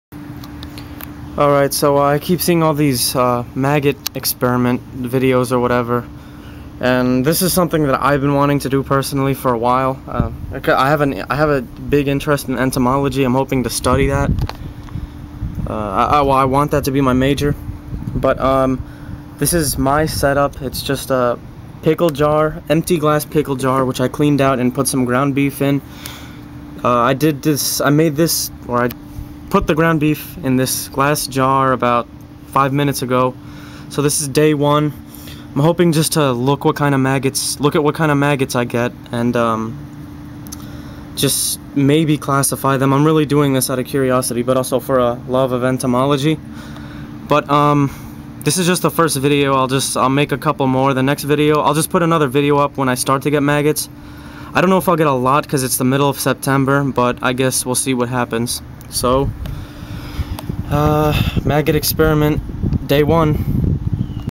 The Odious Maggot Experiment - Day 1
I have been wanting to do this for a while. The next video I will put up wil be when maggots start appearing in/on the meat.

Day 1 of experiment: Sept. 15, 2009